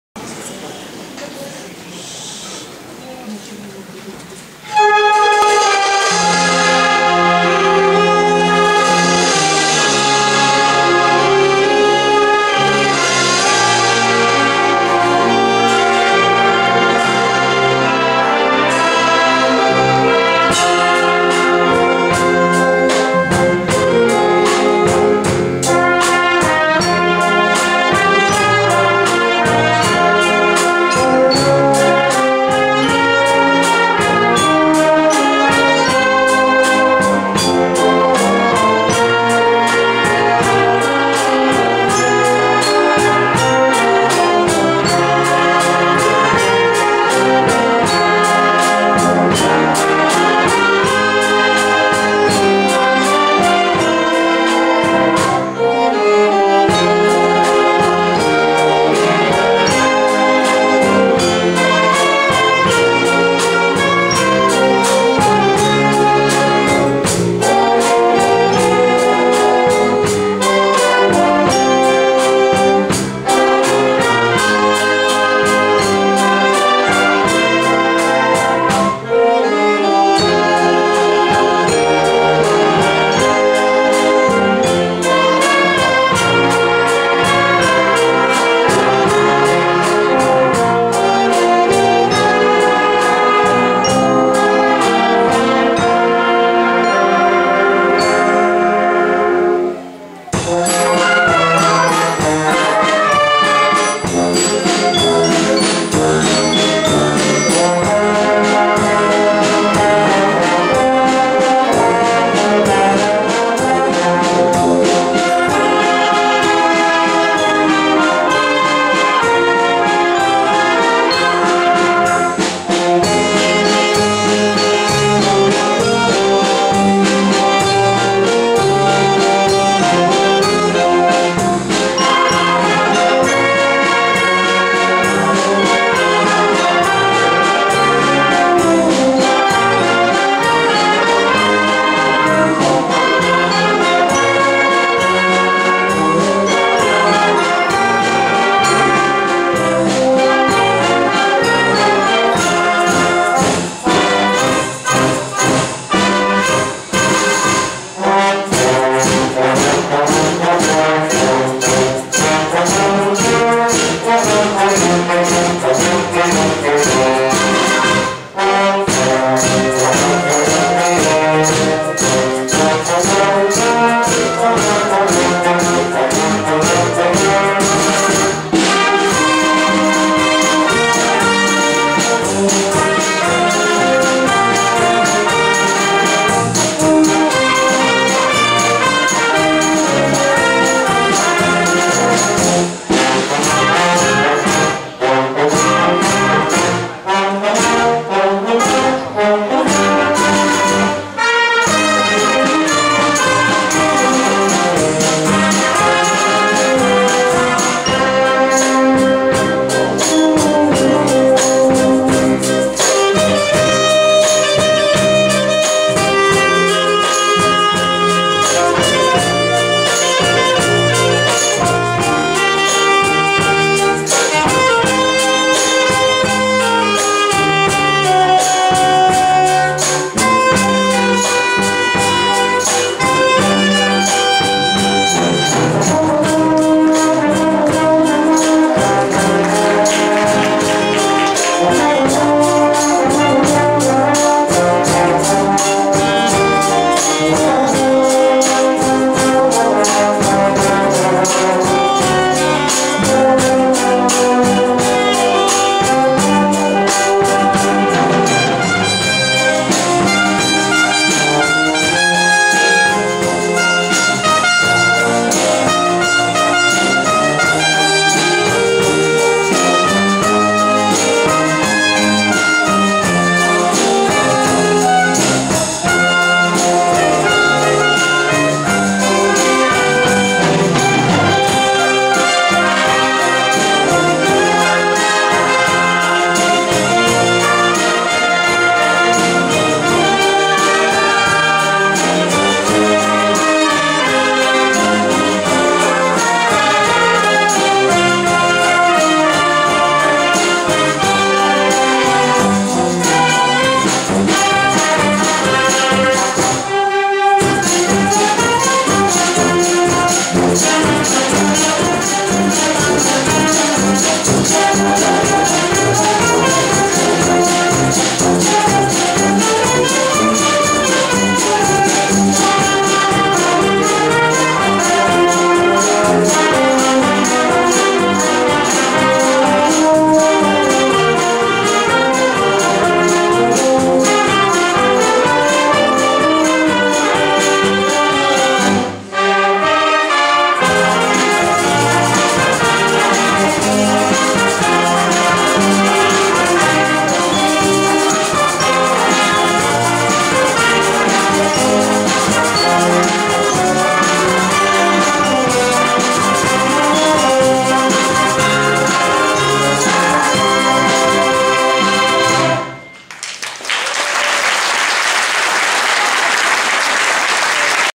ת. כלי נשיפה קונס. ב-ש, מנצח חיים גפטר - מחרוזת ירושלים
תזמורת כלי נשיפה קונסרבטוריון באר-שבע, ישראל
מנצח: חיים גפטר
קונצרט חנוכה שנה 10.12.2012
מחרוזת שירי ירושלים

Wind orchestra of Beer-Sheva conservatorium, Israel
Conductor: Chaim Gafter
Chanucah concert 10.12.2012
Jerusalem songs potpourri